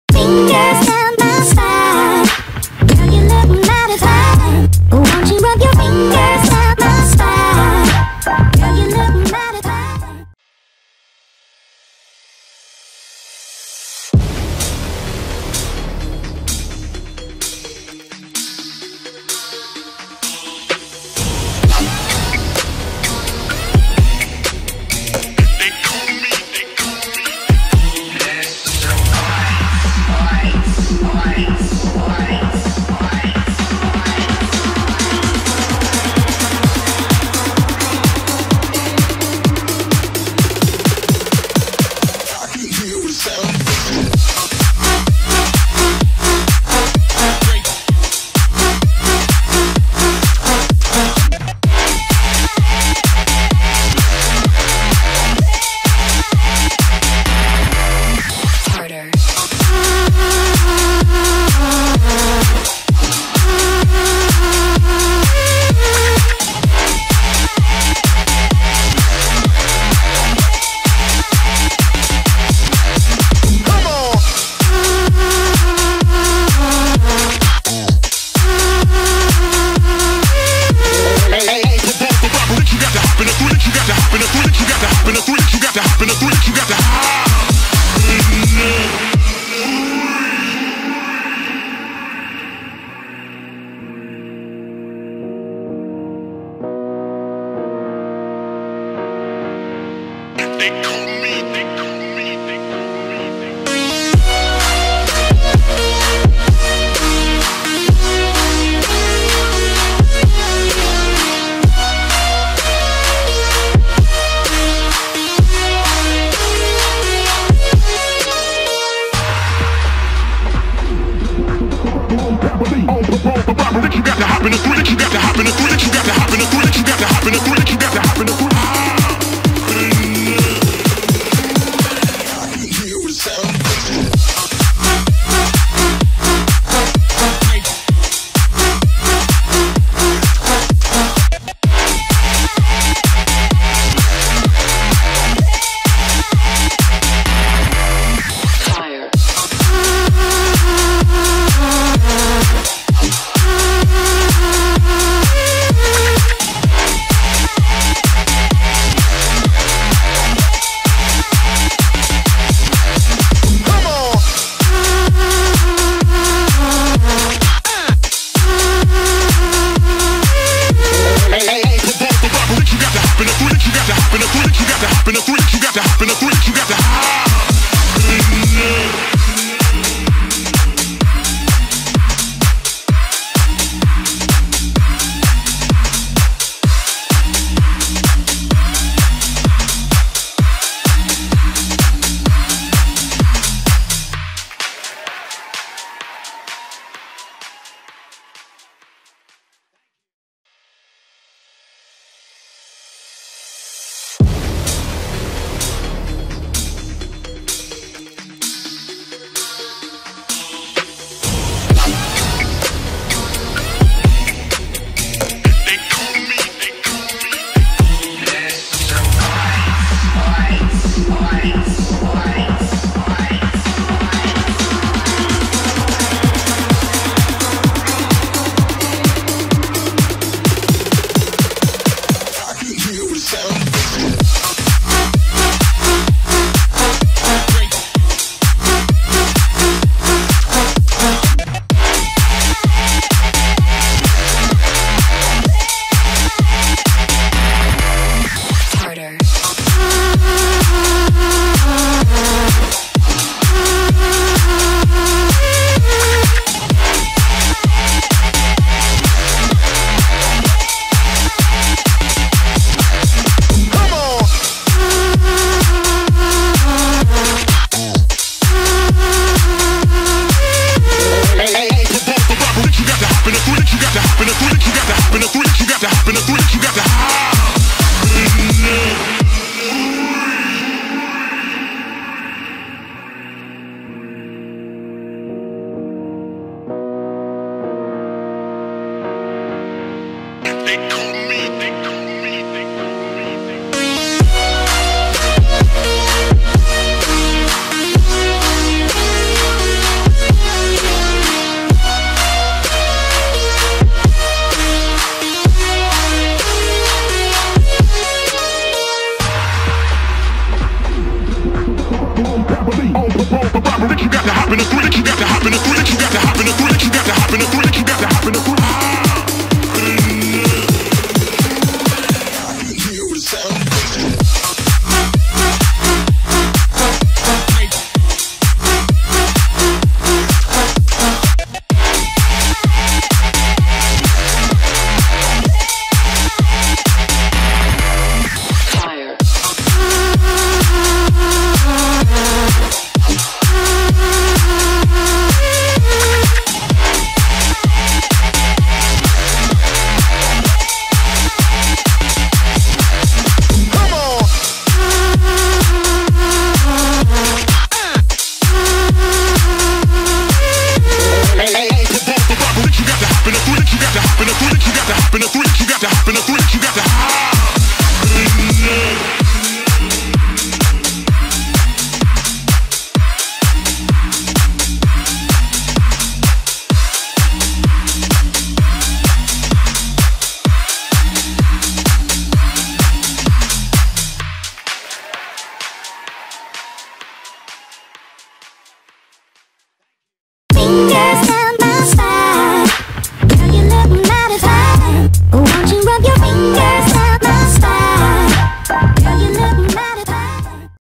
Super car to Bentley Continental GT 2018 launch | YOURCAR
Super car to Bentley Continental GT 2018 launch
The Bentley Continental GT 2018 was completely redesigned, inspired by Bentayga and the 10 Speed ??6 concept. The car uses a 6.0-liter W12 engine, accelerating 0-100 km / h in 3.7 seconds.